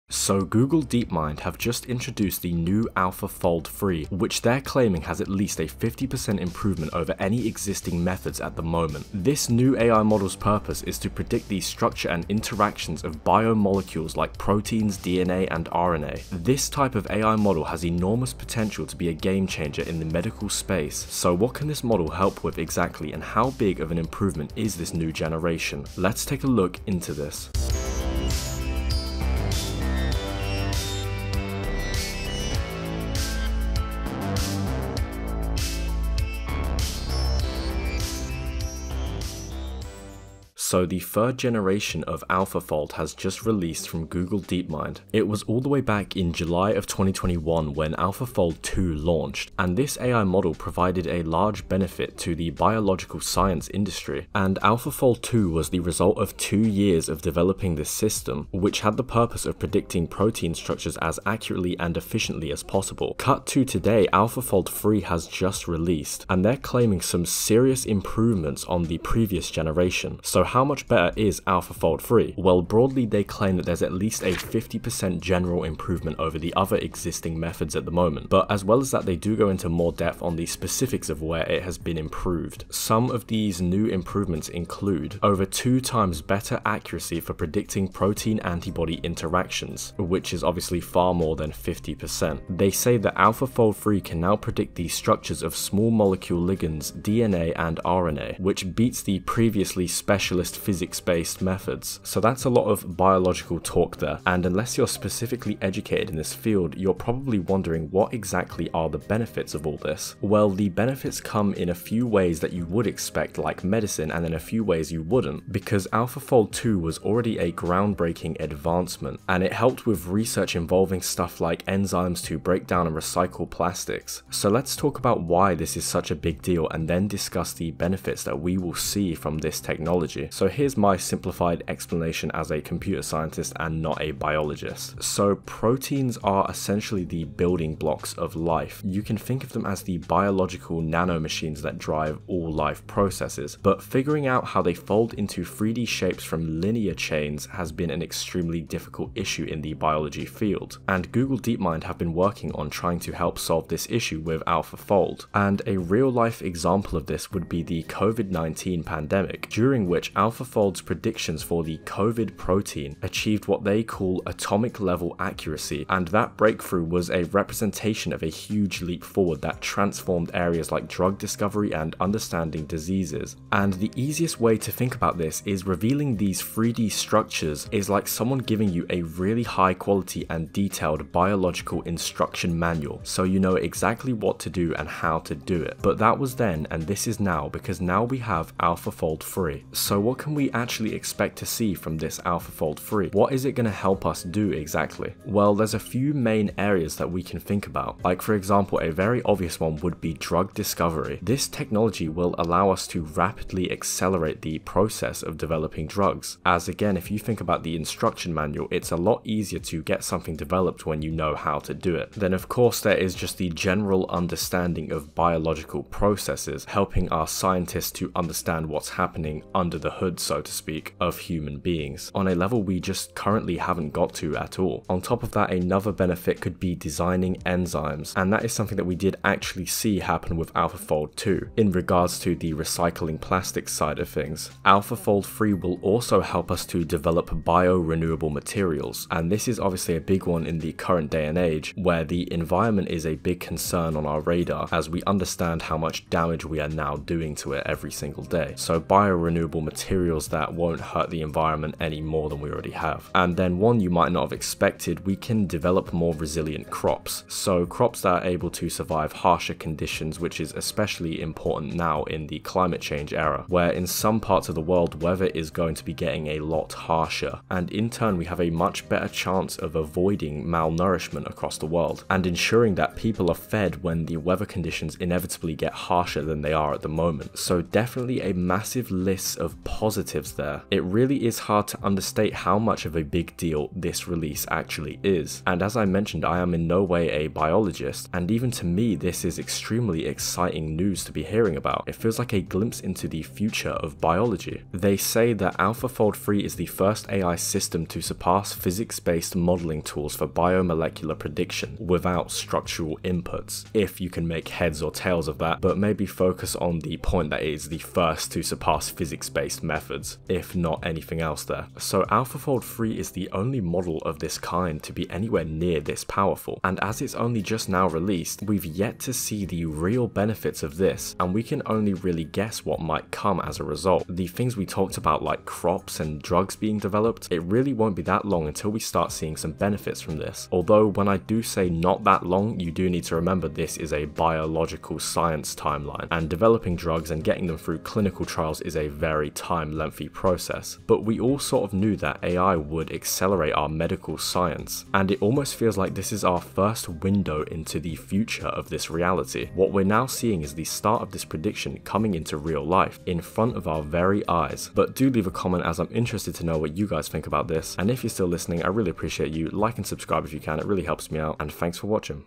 Google Deepmind Announces NEW Ground BREAKING AI Model AlphaFold 3 (A Step Closer To Singularity)
Today we talk about google deepminds new AlphaFold 3 which changes the game for biological science. This truly is our first major step towards the singularity.